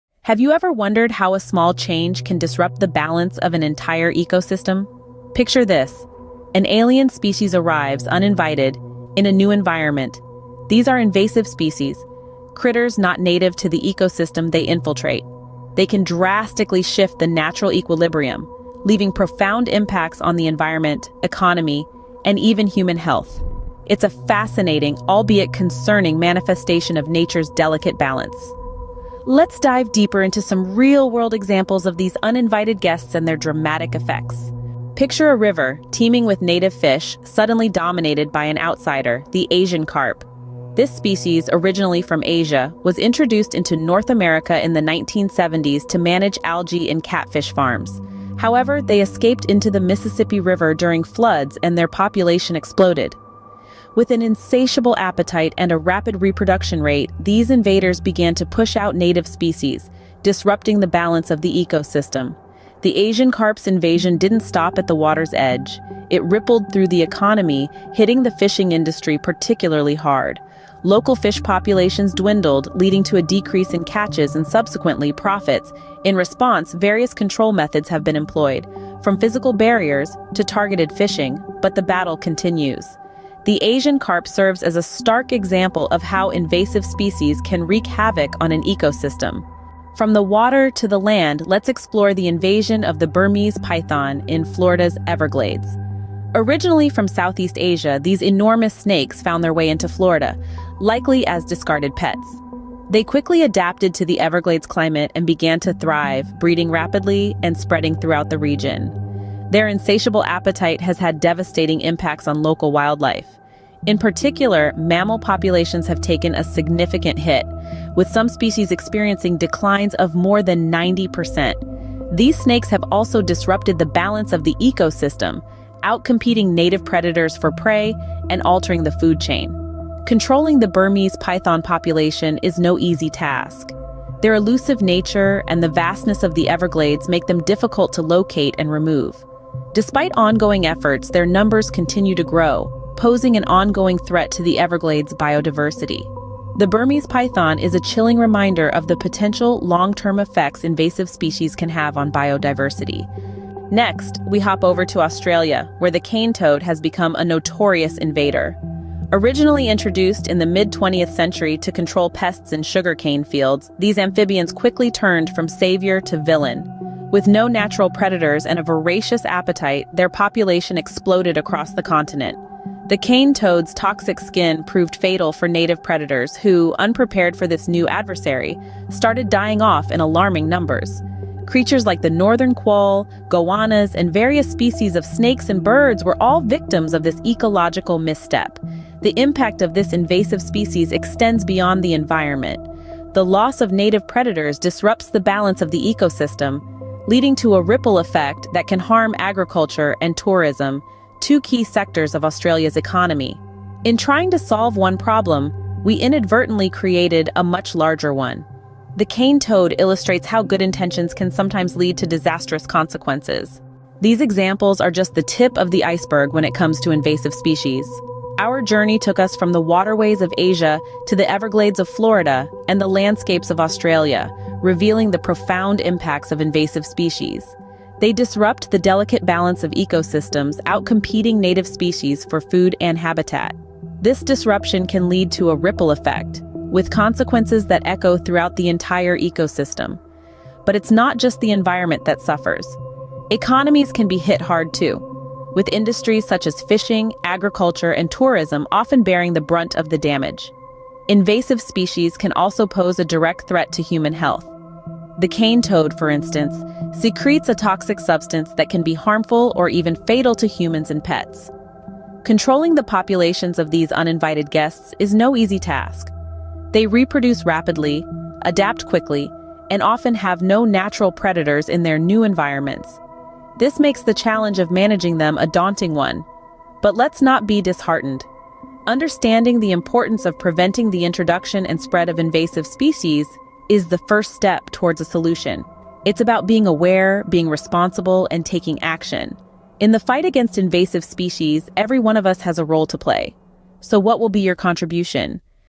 Invasive Species | Educational Videos for Kids | Wiz World Wonders
Get ready to uncover the secrets of invasive species in our latest video adventure! Join us as we dive deep into the world of these sneaky invaders, exploring how they disrupt ecosystems and threaten native wildlife around the globe. We'll reveal the surprising ways invasive species make their way into new habitats and wreak havoc on the environment. But fear not! We'll also shine a light on the innovative strategies scientists and conservationists are using to fight back against these unwanted intruders. With stunning visuals and captivating stories, this video is a must-watch for anyone curious about the hidden dangers lurking in our natural world. Don't miss out on this thrilling journey to uncover the truth about invasive species and what we can do to protect our planet!